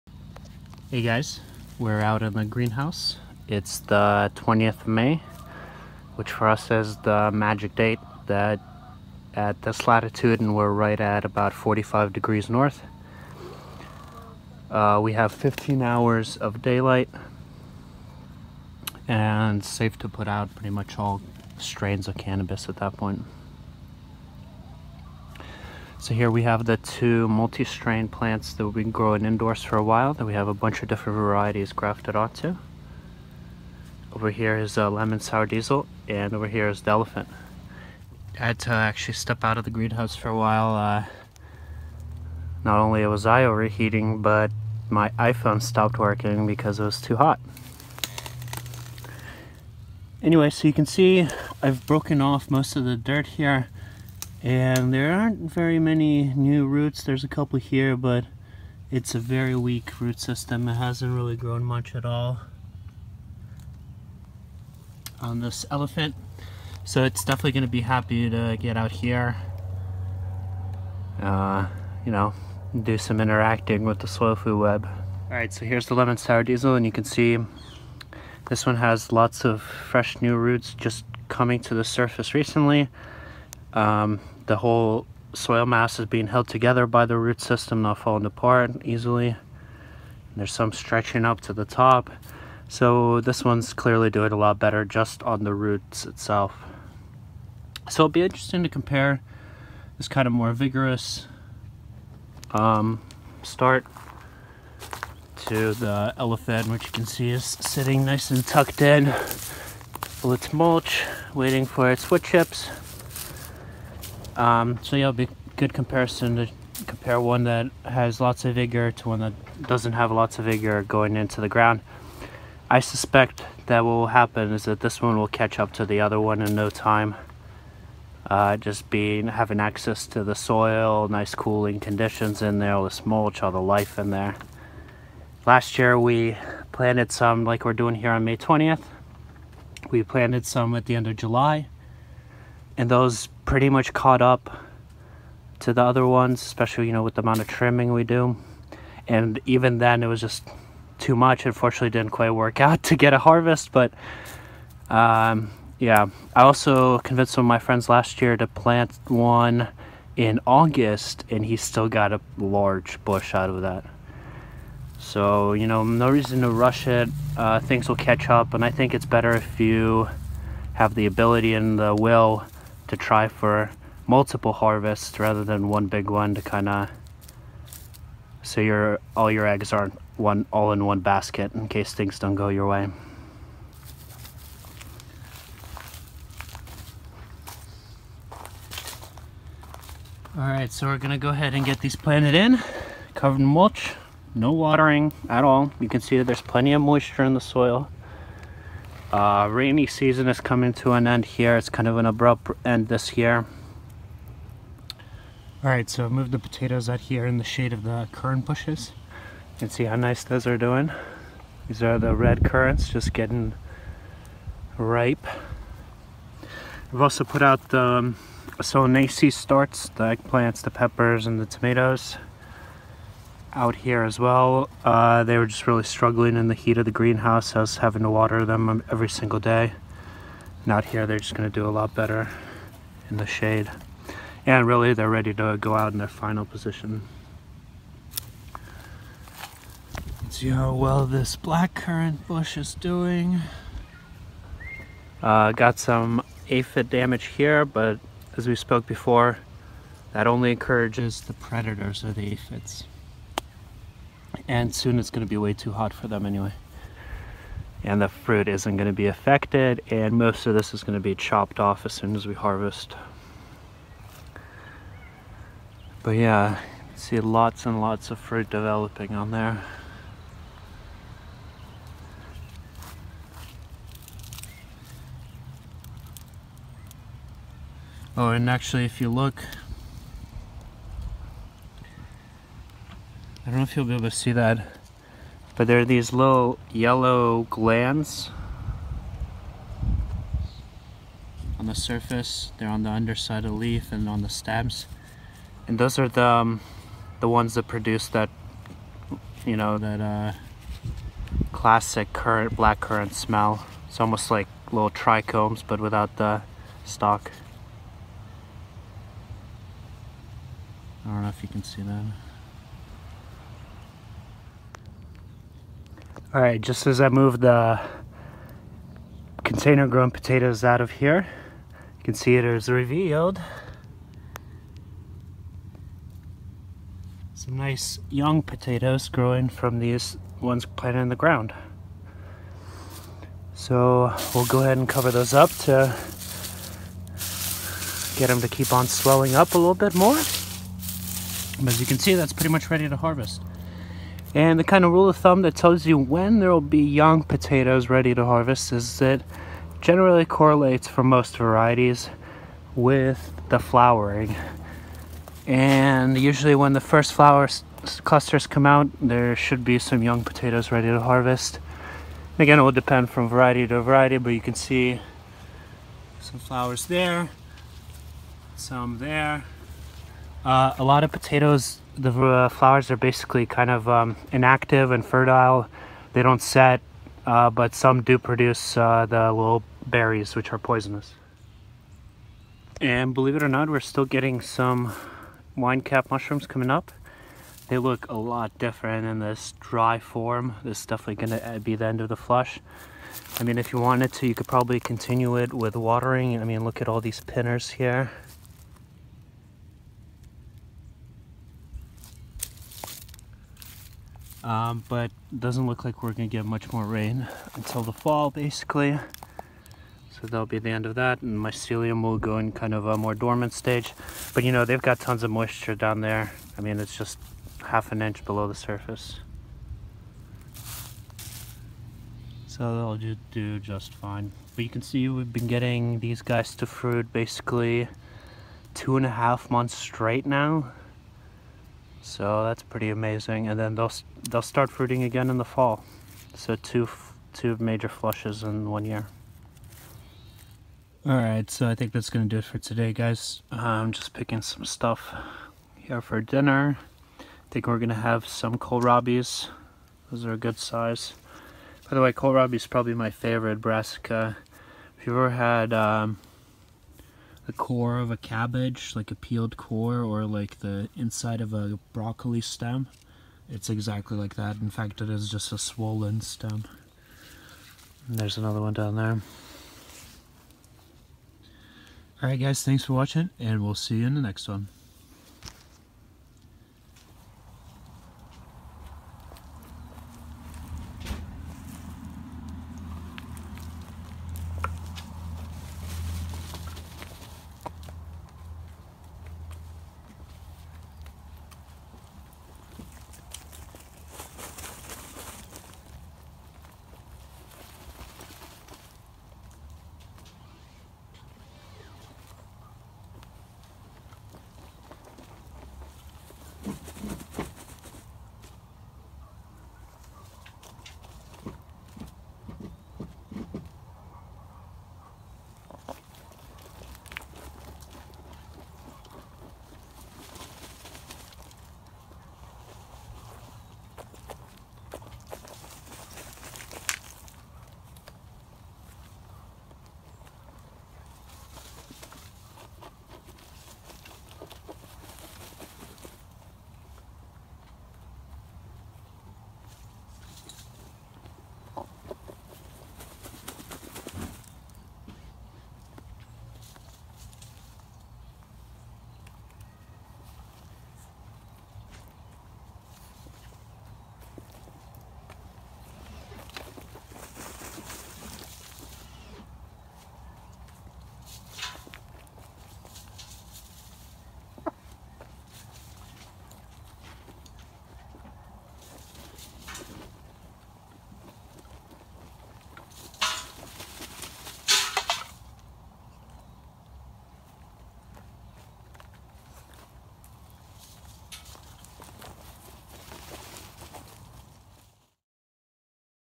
Planting Summer Crop - Episode 70
It was a really hot day when it was time to plant out the summer cannabis crop in the greenhouse and the plants were eager to get into the ground!